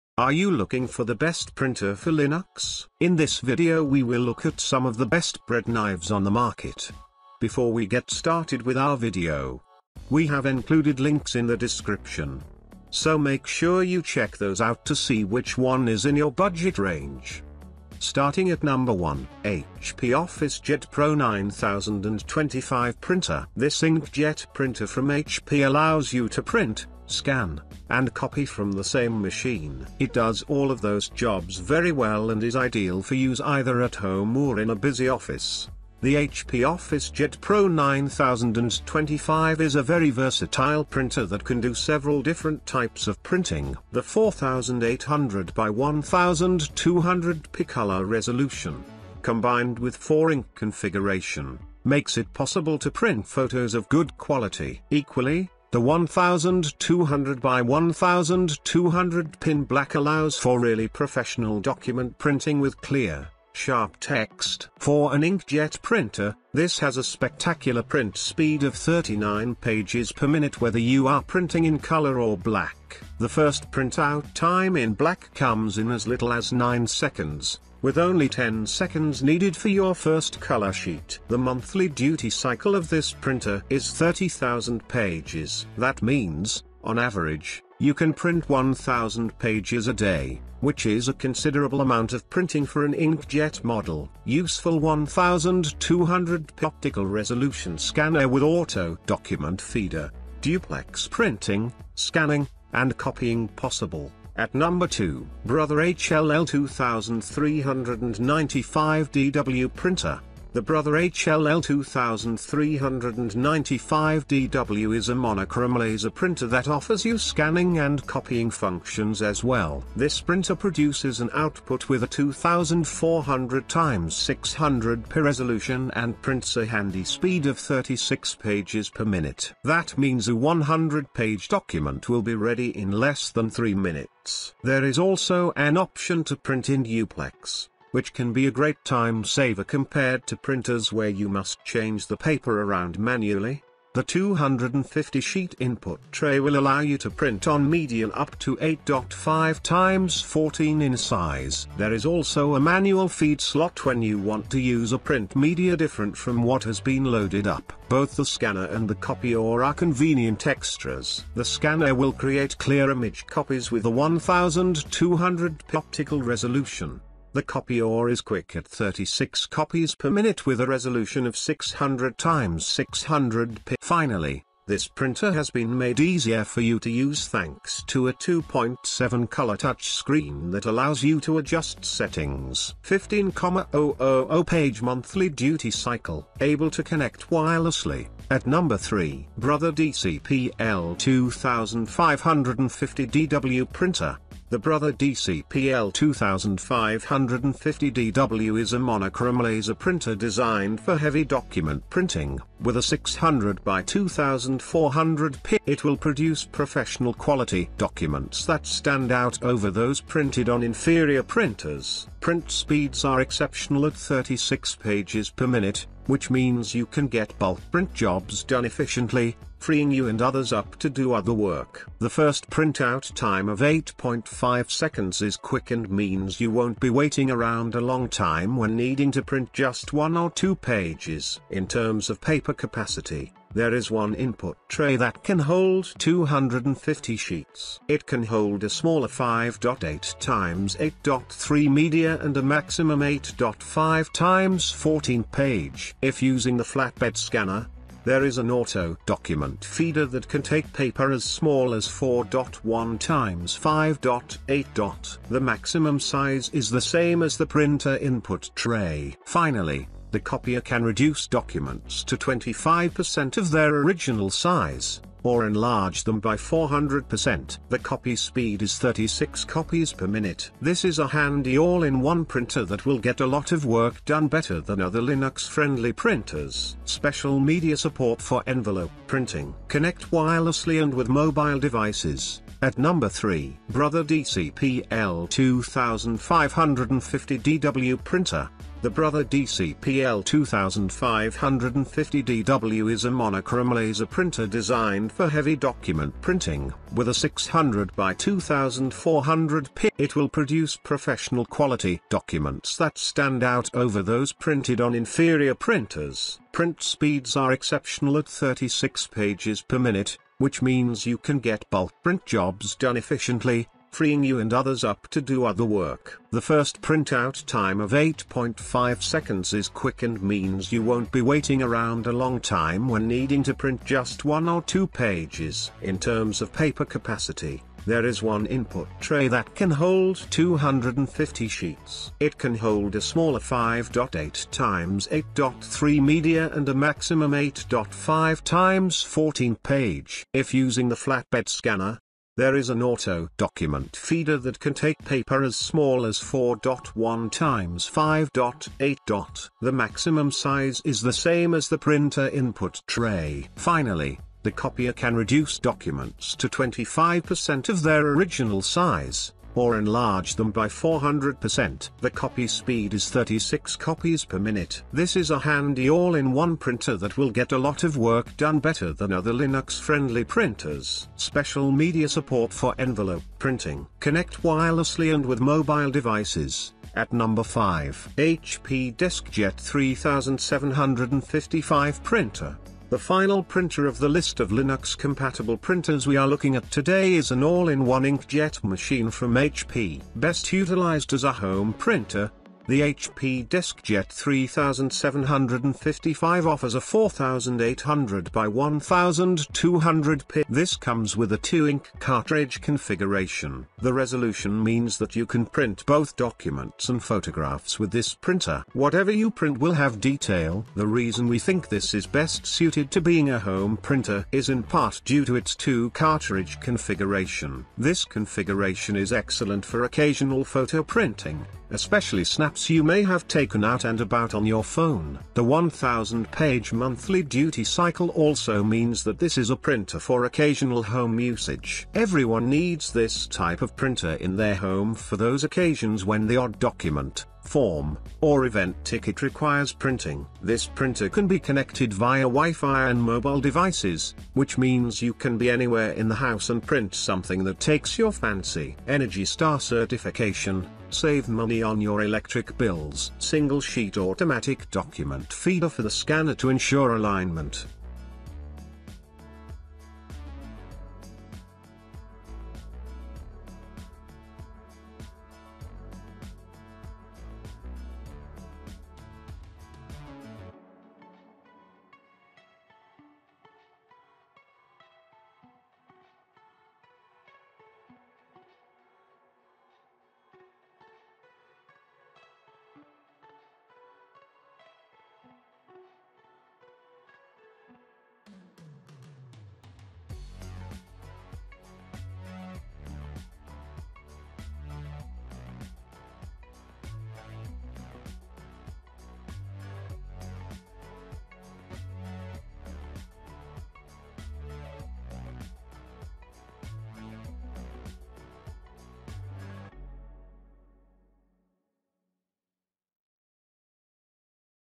✅Top 5 Best Printer For Linux 2021 With Buying Guide
1. HP OfficeJet Pro 9025 Printer.

2. Brother HL-L2395DW Printer.

3. Brother DCP-L2550DW Printer.

4. HP Color LaserJet Pro M479fdw Linux.

5. HP DeskJet 3755 Printer.

Hey guys in this video we are going to be checking out the Best Printer For Linux. You Can Buy Right Now. We made This List Based On Our Personal Opinion and hours of research & we have listed them based on the type of features and Price. We have Included options for every type of user so whether you are looking for the best budget.

      owners.